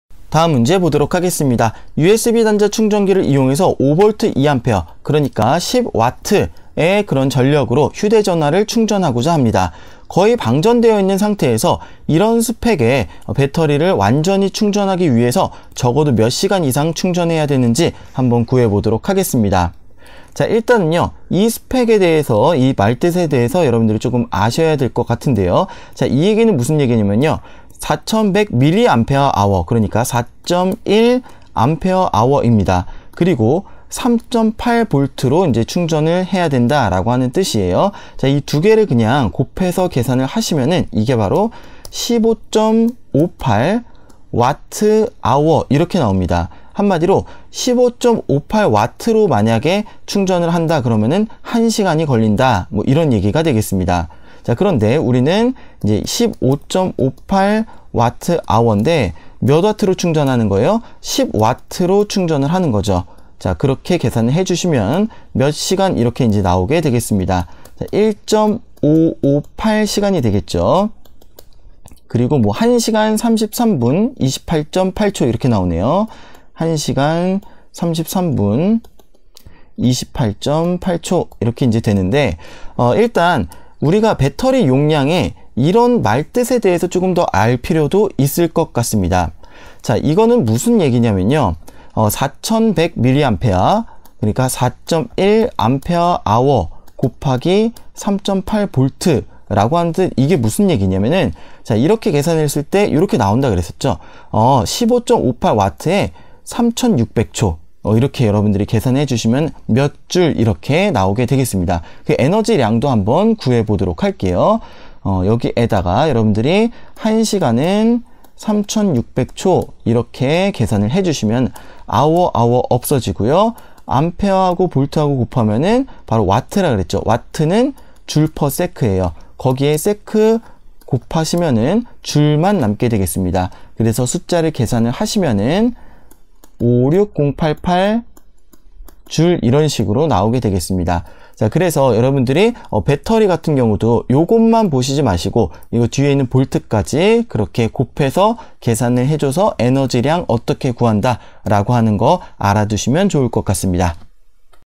열역학 (연습문제 2-15) 배터리 용량과 충전시간
열역학 쉽고 빠른 요약 강의 영상입니다.
SuperBrain의 강의 채널입니다.